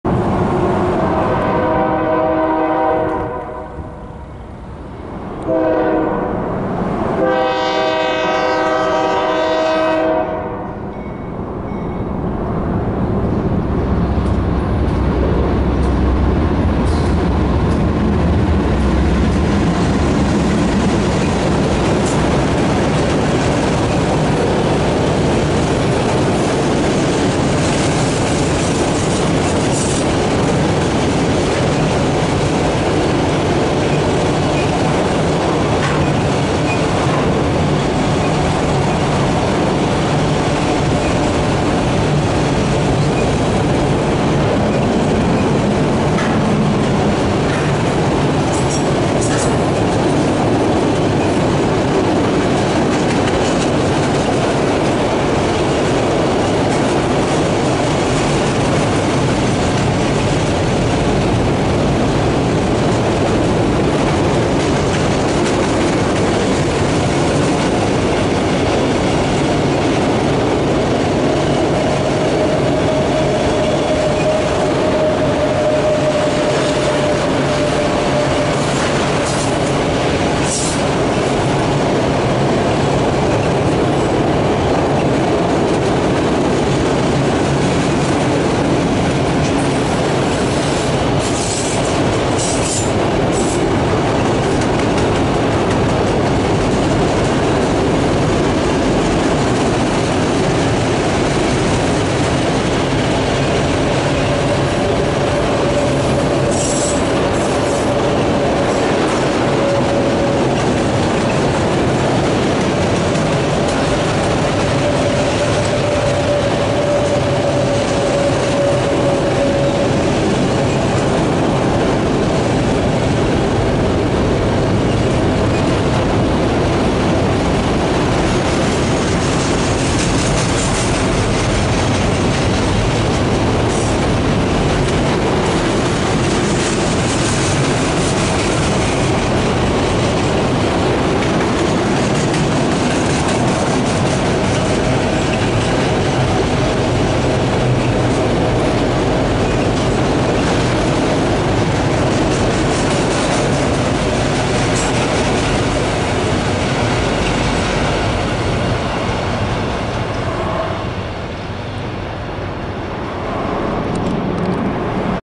Railfanning - BNSF 4090 - Chesterton, IN (3-3-18)
Railfanning when BNSF 4090 passes Chesterton, Indiana.

Train set

Train set